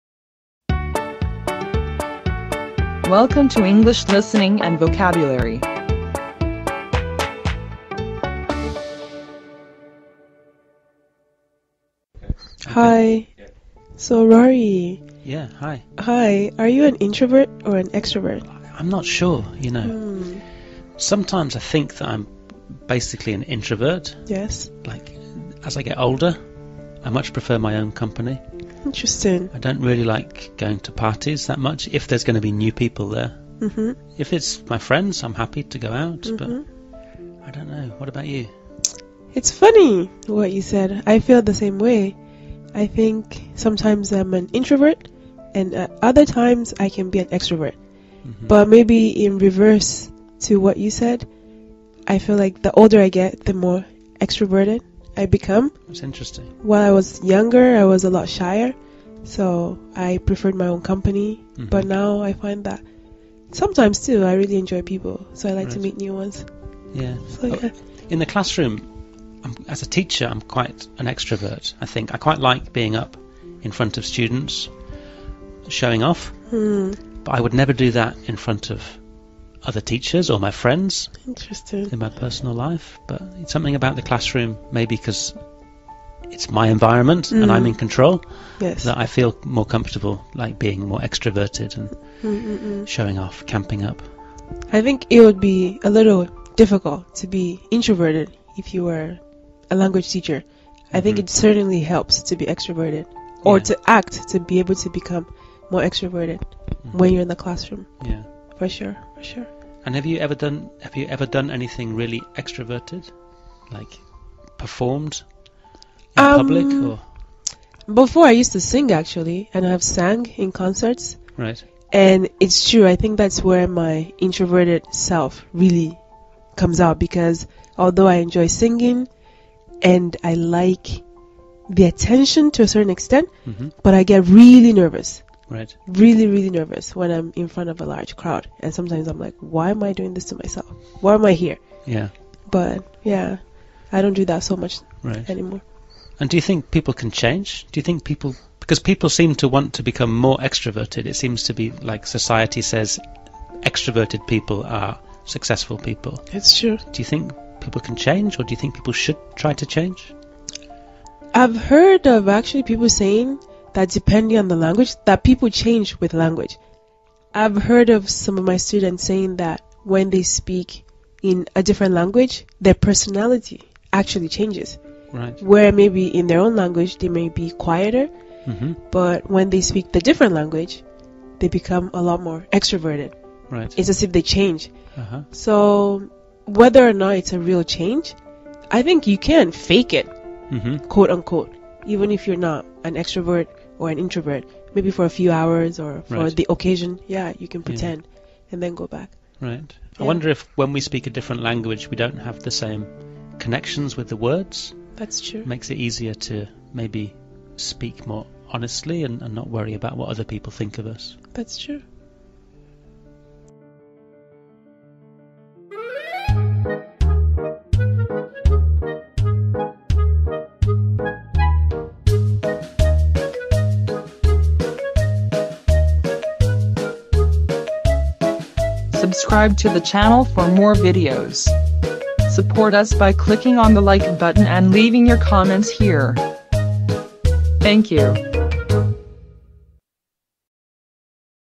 Introverts and Extroverts 👉 English Listening and Vocabulary ✅ Level 7
A short story helps to improve your English Vocabulary and Listening Skills.
Used images are from unsplash.com, and pixabay.com.